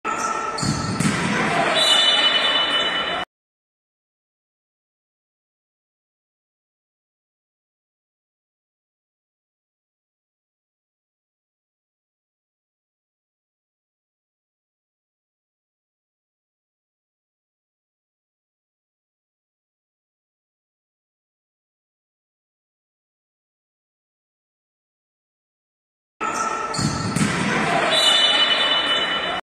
Video 2084 Fallos en la Lectura del Juego
Dadas las consideraciones, se comete una infracción de mano y la árbitra pudo haber otorgado una ventaja hacia las jugadoras amarillas.